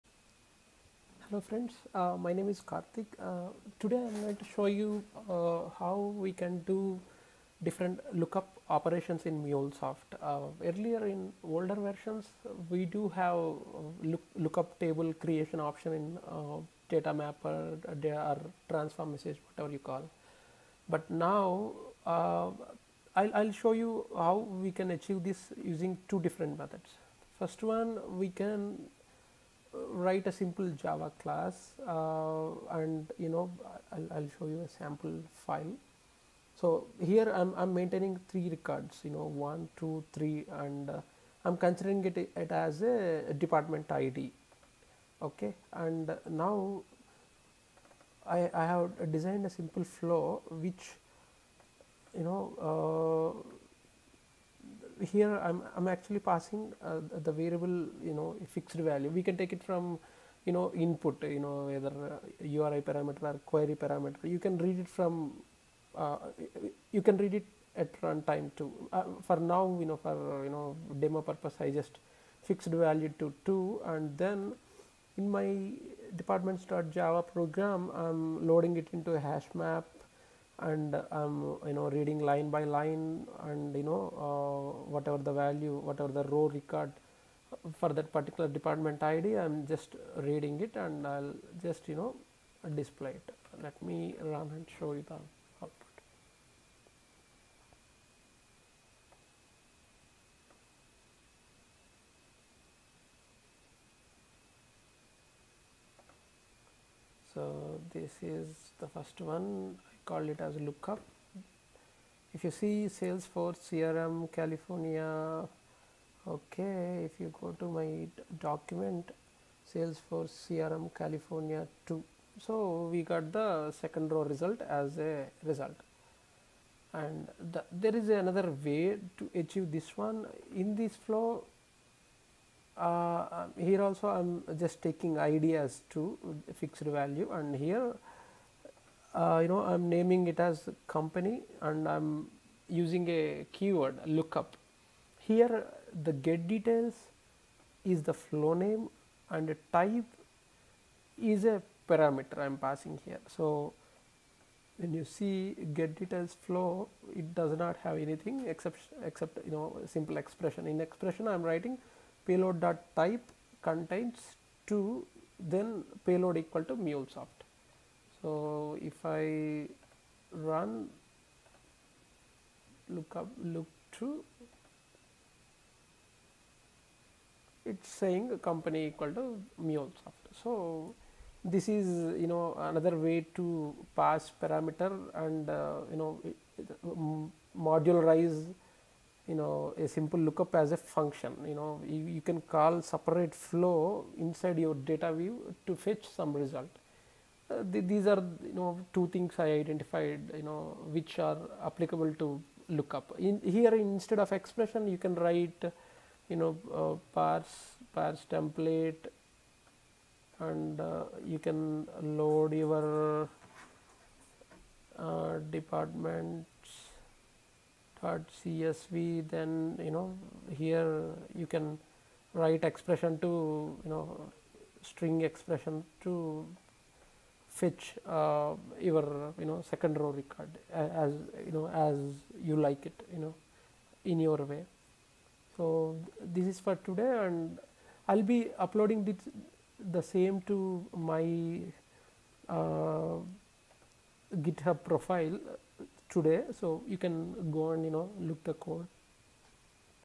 MuleSoft Look up function
A simple Mule application to do look up function in MuleSoft.